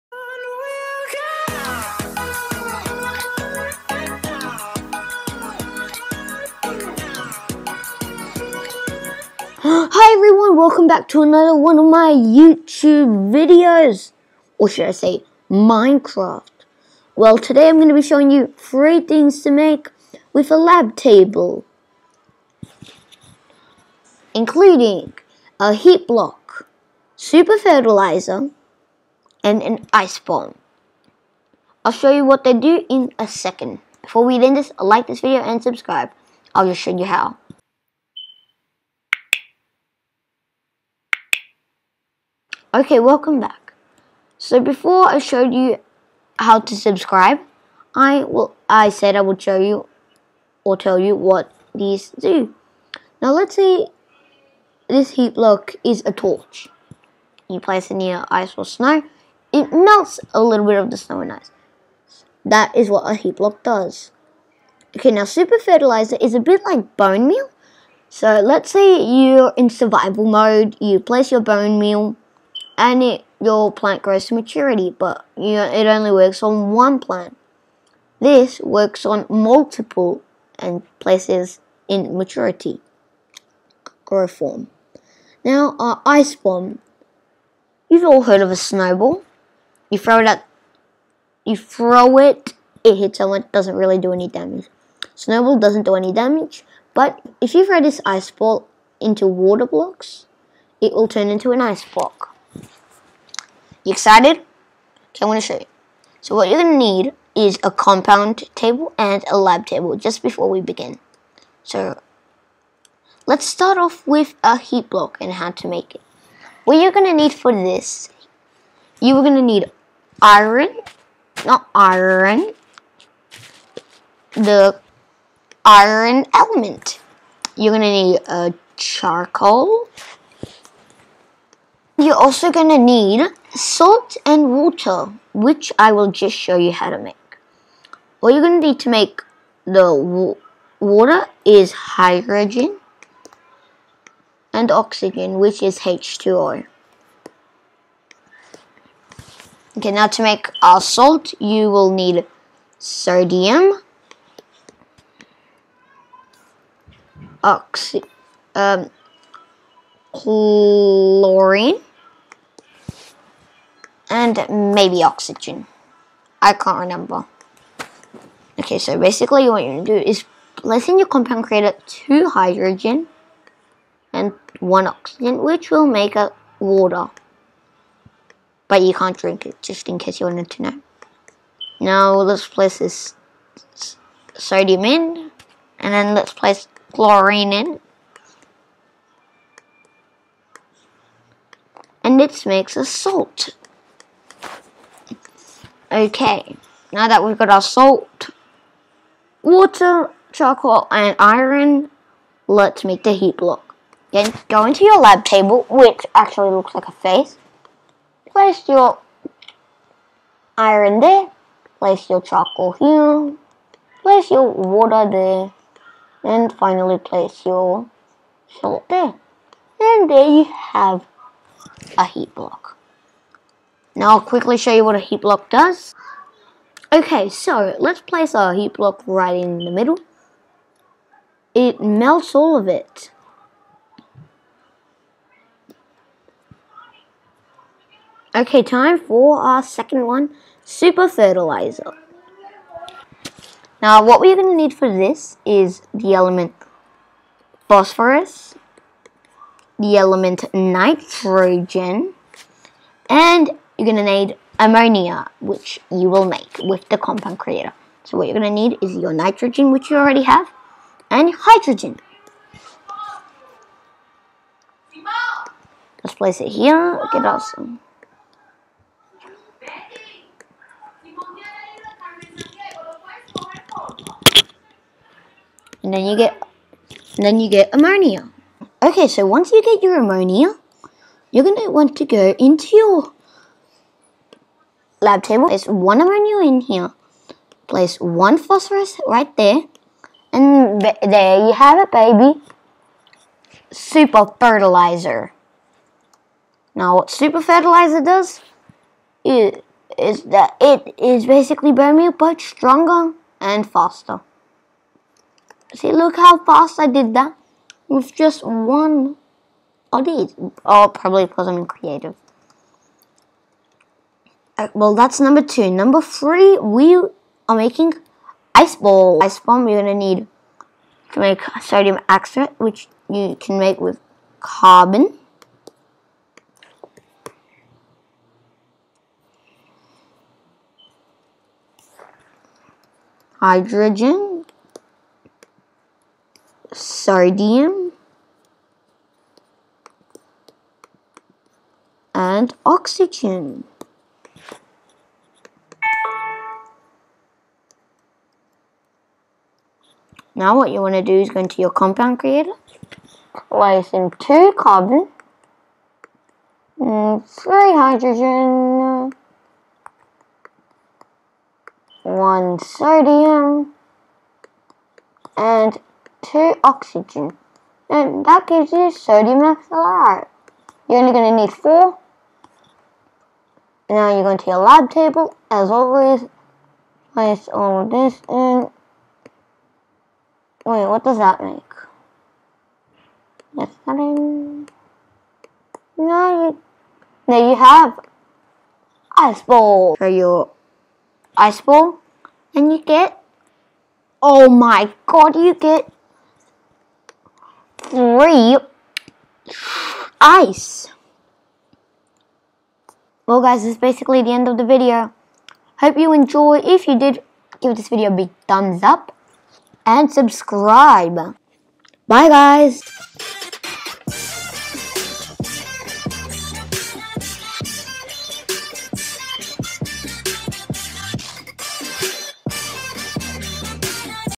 3 things to make with a lab table
this video well show you how to. make ice balls ,super fertiliser and heat blocks